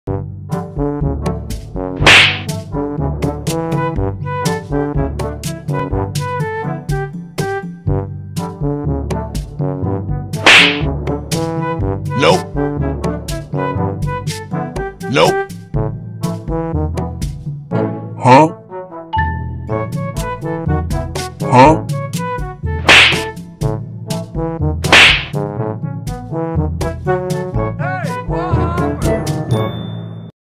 PUBG AND FREE FIRE BANNED? It is not opening in IPhone and ipad trolling video
PUBG and free Fire big games are not opening iPhone and iPad iOS devices. Is it bug? Anyone facing this issue